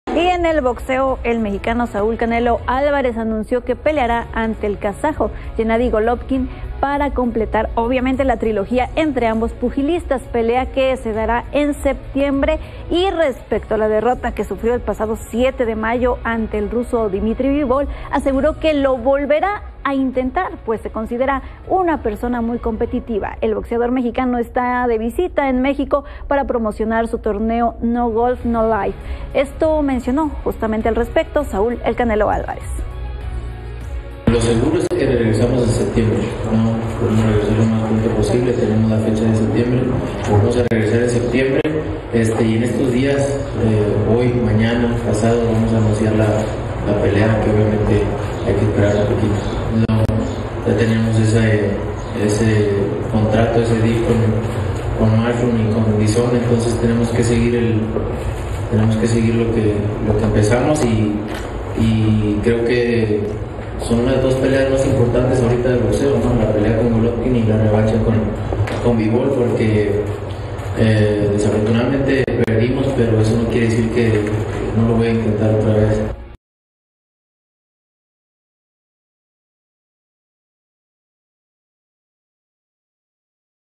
Canelo Álvarez confirma tercera pelea con Golovkin y revancha ante Bivol
El campeón indiscutido mexicano inauguró su torneo de golf y también habló sobre su futuro en el boxeo